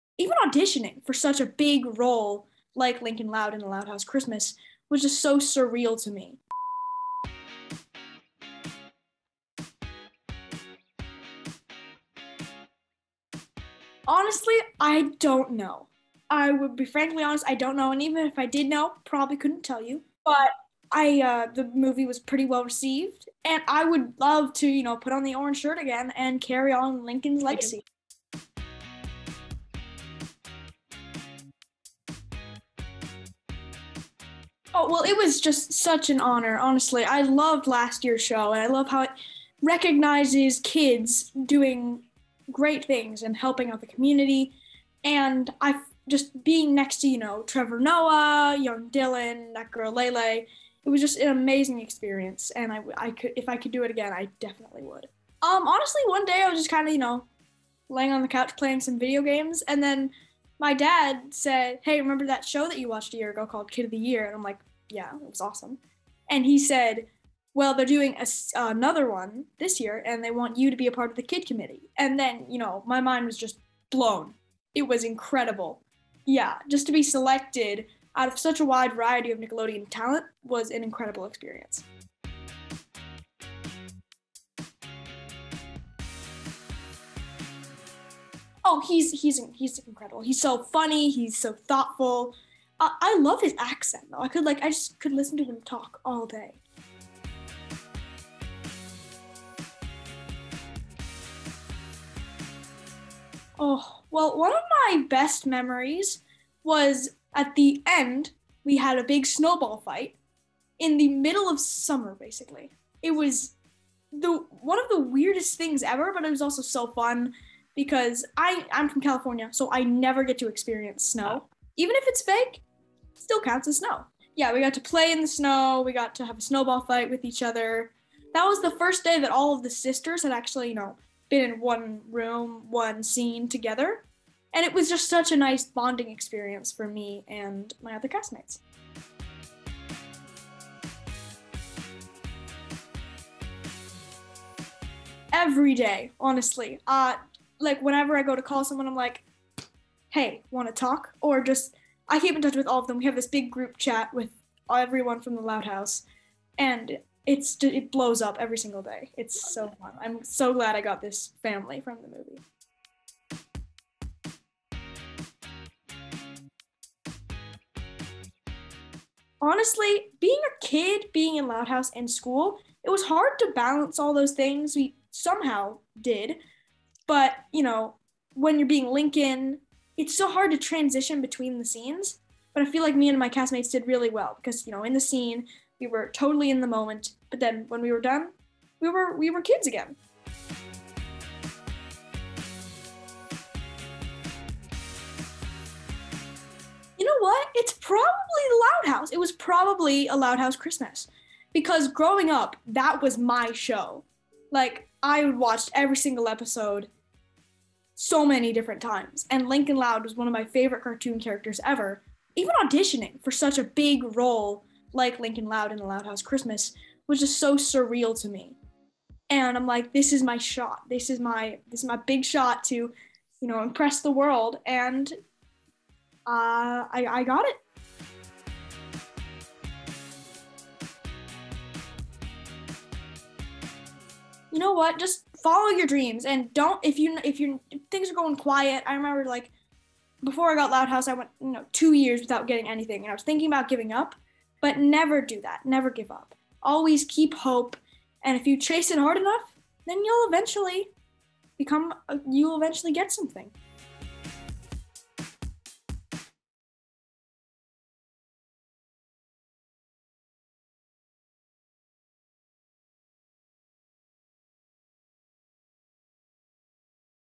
Wolfgang Schaeffer ‘Lincoln Loud’ Talks Nickelodeon & TIME’s Kid of the Year Award & More!
Nickelodeon star Wolfgang Schaeffer (‘Lincoln Loud’) movie ‘A Loud House Christmas,’ talks 2022 Nickelodeon ‘Kid of the Year’ Award, working with Trevor Noah & more!

TIMESTAMPS:
00:00 Intro
00:30 Nickelodeon’s kid committee member, for 2022 KID OF THE YEAR AWARD!
02:25 KEEPING IN TOUCH WITH ‘A LOUD HOUSE CHRISTMAS’ CAST!
03:13 MOST MEMORABLE AUDITION EVER!
03:53 TIPS for other young people who want to become ACTORS!